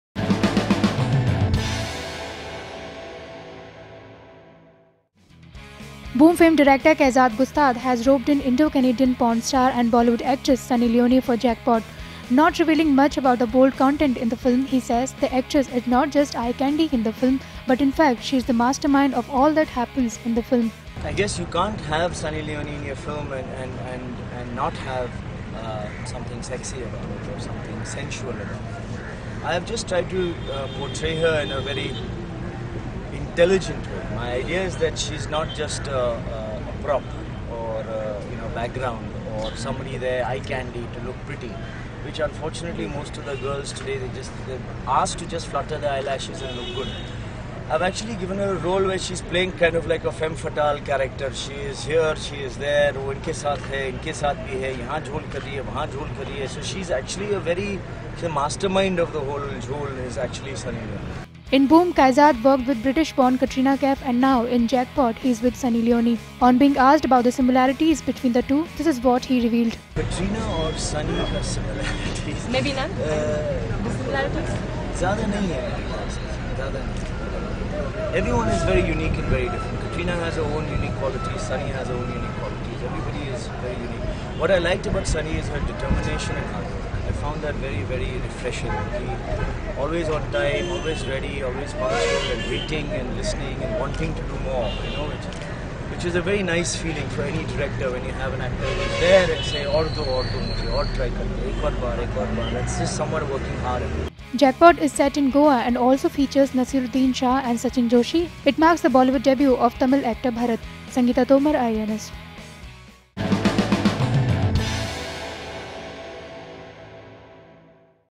Sunny Leone not just an eye candy in Jackpot
'Boom' fame director Kaizad Gustad, who has roped in Indo-Canadian porn star and Bollywood actress Sunny Leone in Jackpot, says she is not just a prop in the film.